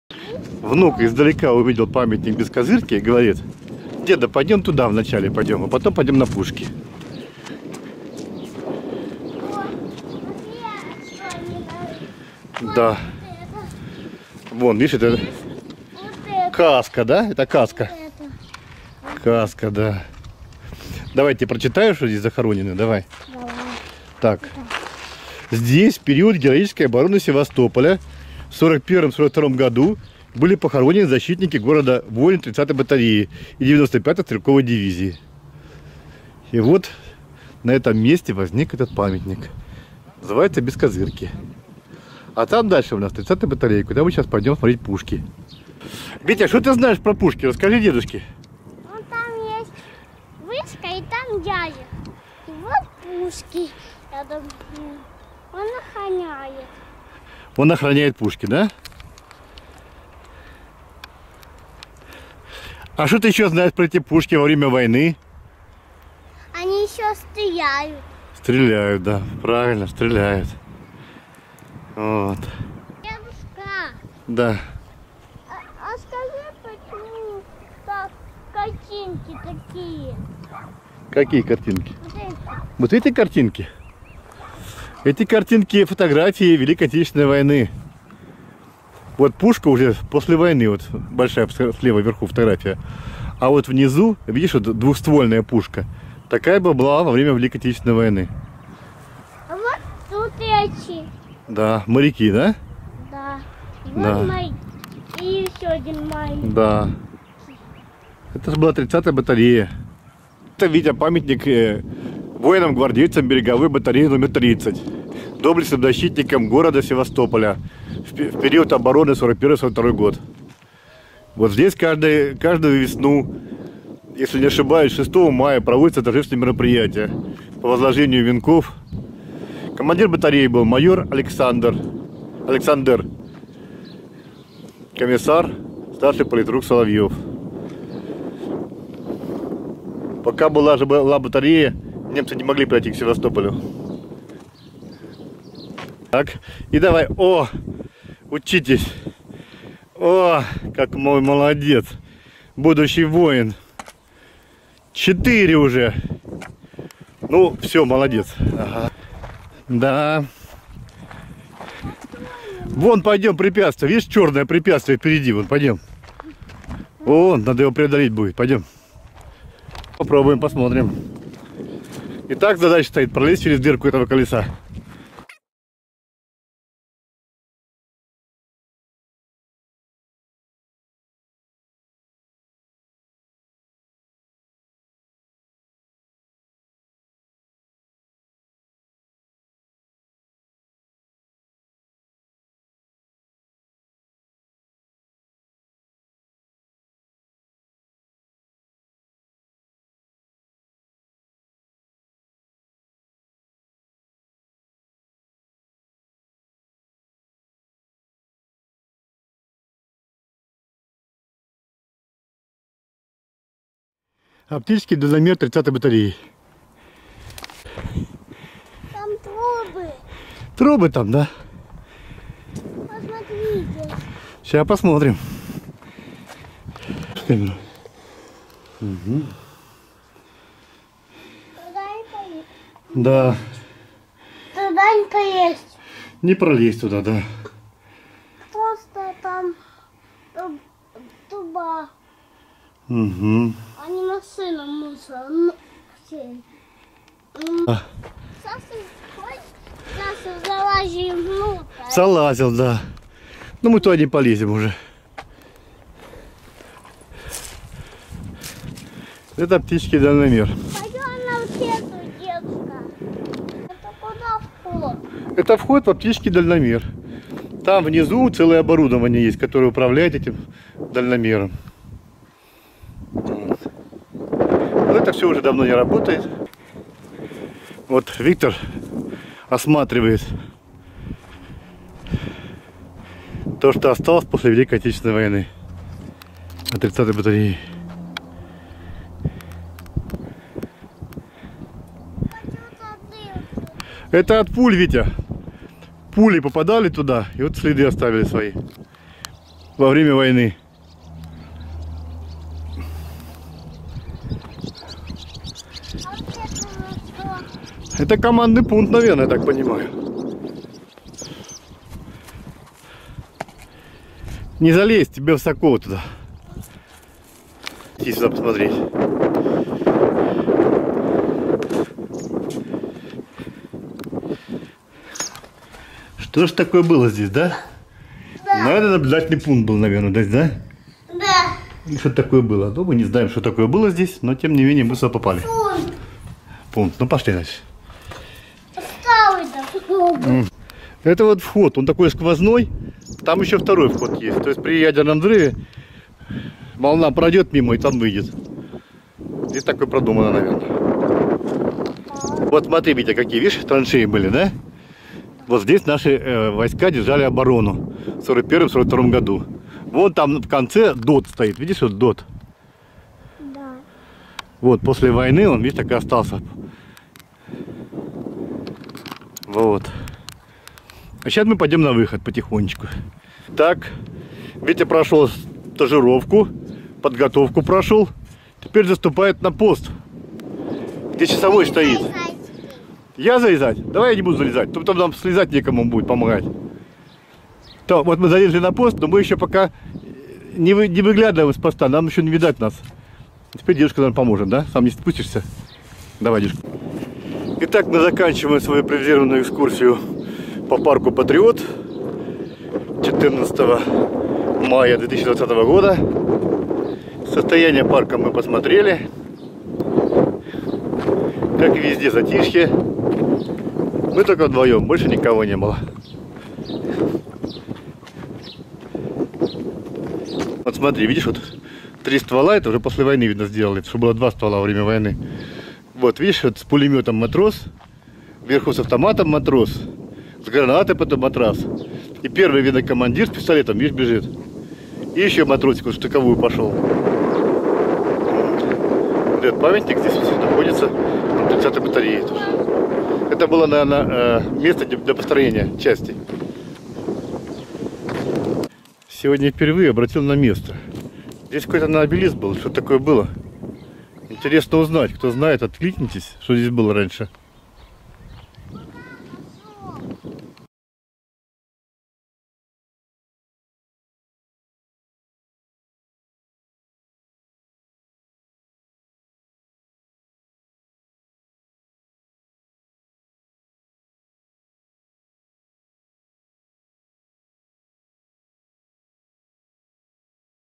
30 я бронебашенная батарея и парк Патриот чере 2,5 года после открытия
Практически забытый и заброшенный парк Патриот. Начиналось все с помпой и красиво. Много обещаний  и отсутствие финансирования.